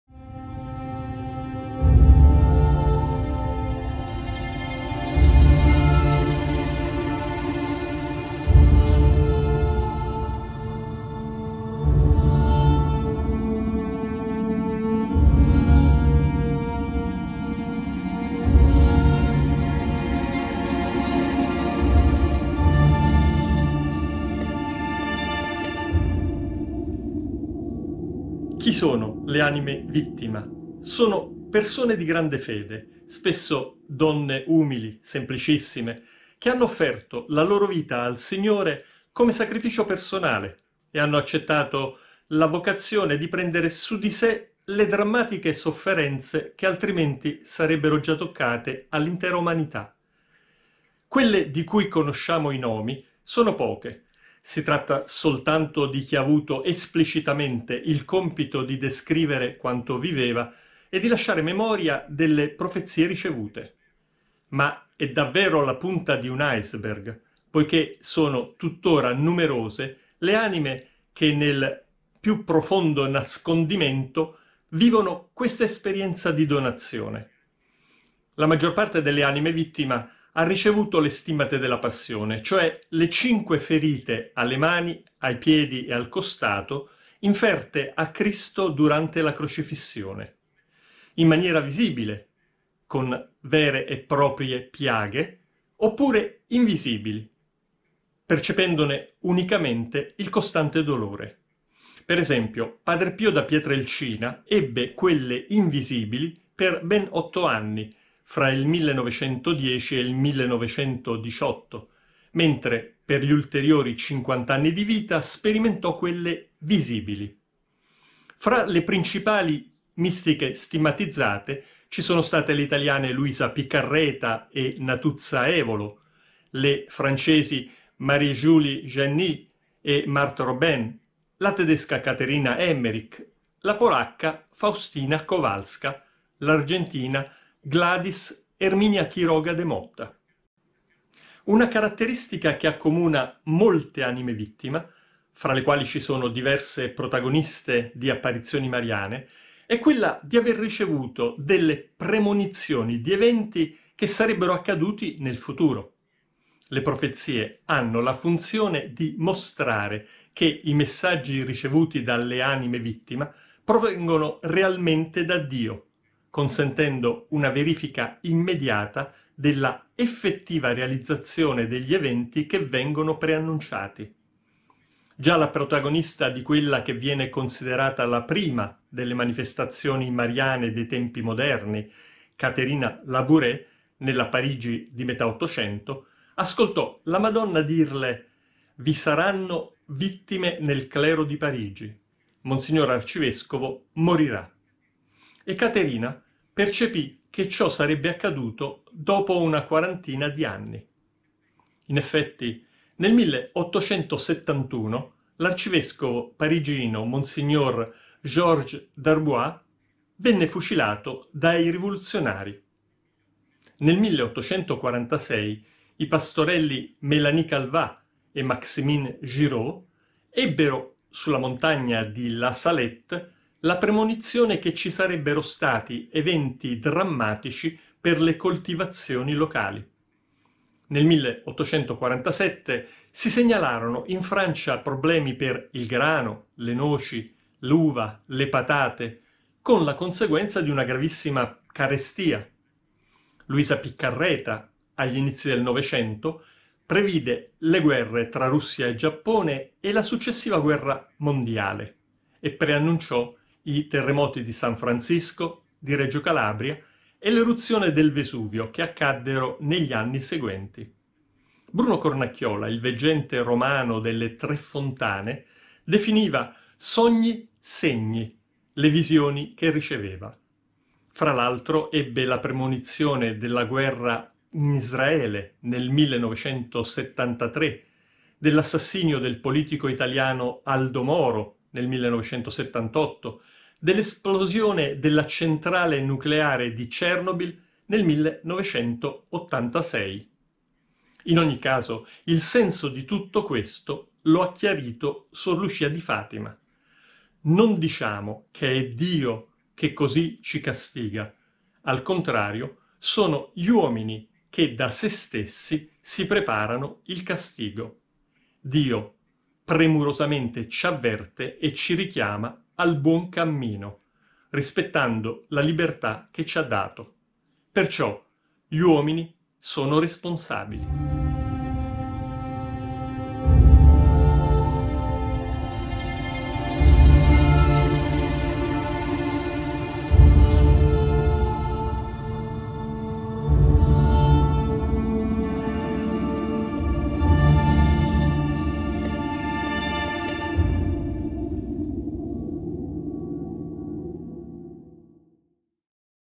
PROROCTWA DUSZ OFIARNYCH - Co przewidziały? I Saverio Gaeta
ZOBACZ KSIĄŻKĘ "DUSZE OFIARNE. HISTORIE DUSZ, KTÓRE RATUJĄ ŚWIAT"

Kim są dusze ofiarne? Do czego powołał je Bóg? Jakie proroctwa wypowiedziały? Co nas czeka? Odpowiada włoski watykanista Saverio Gaeta!